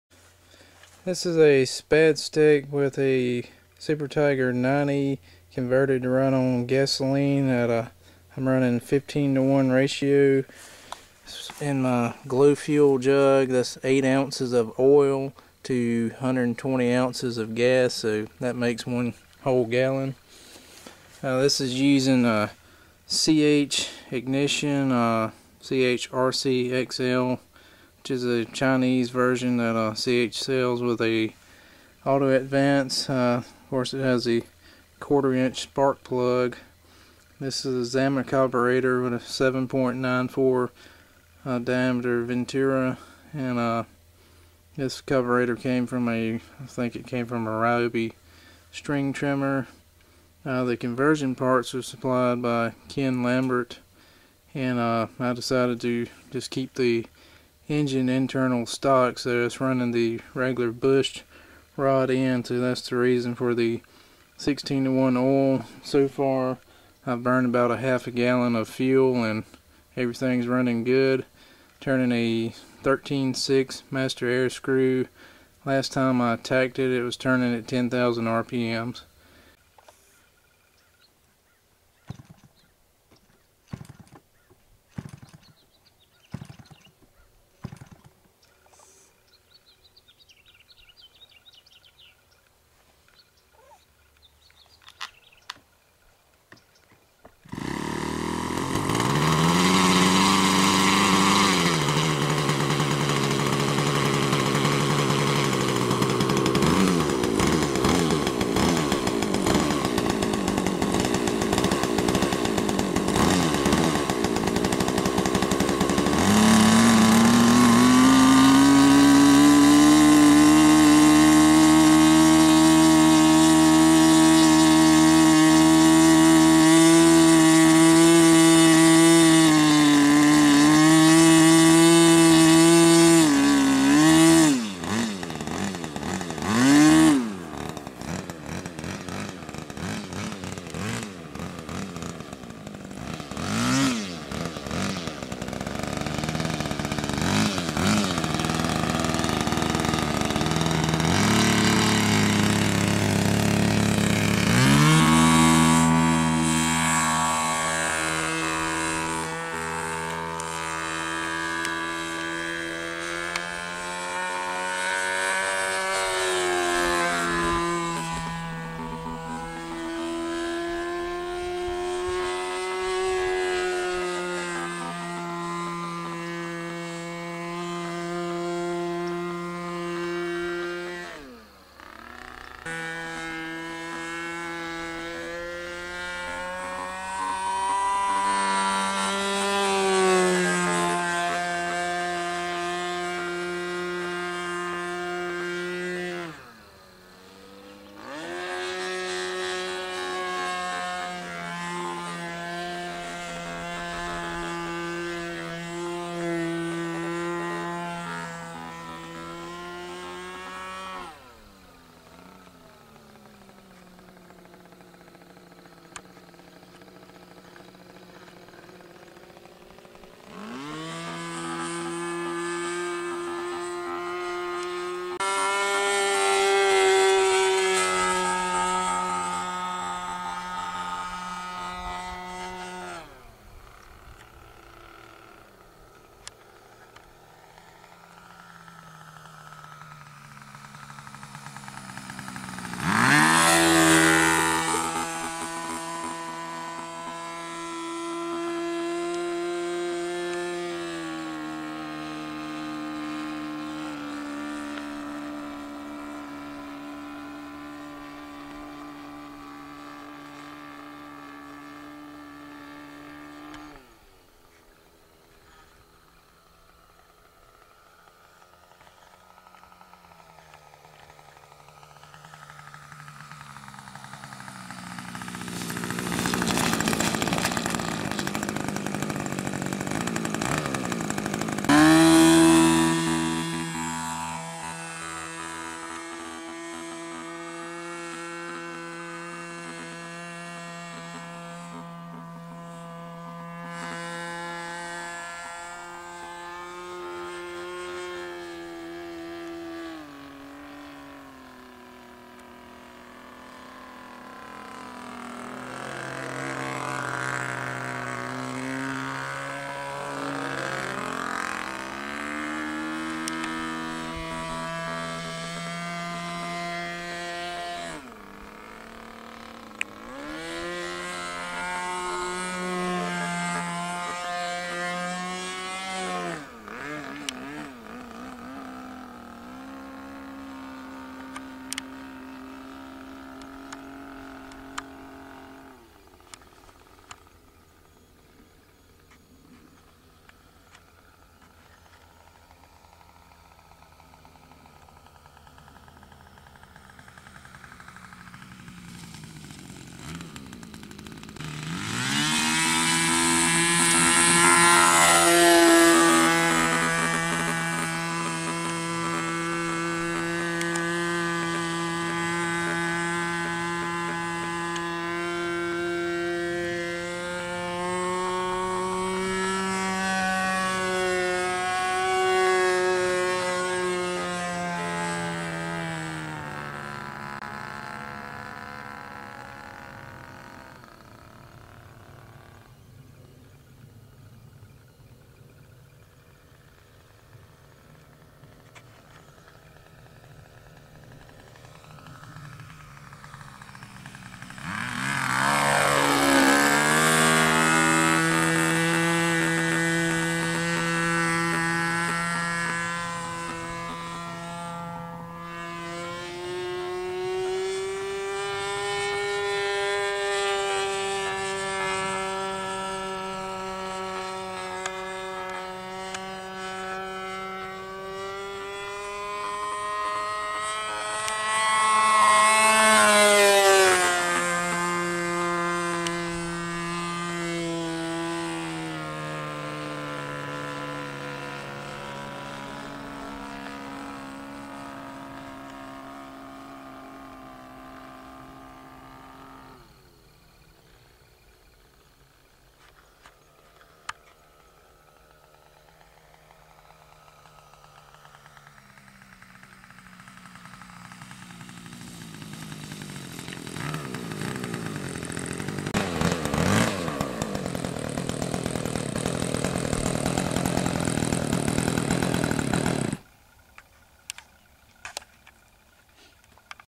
Spad supertigre 90 gas
Modified Spad stick with Super Tigre G 90 converted to run on gasoline.
The only modifications to this engine include: carburetor adapter plate, timing ring, rimfire sparkplug, crankcase cover tapped for pulse line, Some of the exhaust flange on the front of the engine ground down so carburetor  will fit. The rest of the engine is stock. 15:1 oil seems to be just right for the bushed connecting rod. I have talked to a few people running these on 16:1 with no problems. The smallest I know of running like this is a super tigre 51.

I am using Penzoil aircooled 2 cycle oil. Do not use a cheap boat oil. Sthil makes a good synthetic oil that is supposed to be good as well. You need a good quality  2cycle aircooled oil since these engines  run pretty hot compared to boat engines.

Plane specs:
Wing Semi symmetrical 60 X12.5= 750 sq inch
 2 inch Ailerons   108 sq inch
H stab area 135 elevator 35 sq in
V stab area 67.5 Rudder 18 sq in

weight  7.5 to 8.5 lbs guess.

The engine has a CH ignitions RCexel ignition which is from china. It has auto advance built in. The engine uses one of those $20 ¼ rim fire spark plugs! I have a zama carb on it from a ryobi engine and it runs well. Last time I tached it I got 10K on a MAS 13X6.

I am running 15:1 oil or 120 ounces gas and 8 ounces oil to make a gallon. Put in the 8 ounces oil 1st and fill the empty glow fuel jug up with gas! It does spit its share of black oil but hey its running on cheap gasoline and an 8 ounce tank on a 90 2 stroke! I know I have flown over 10 minutes and had about ½ tank left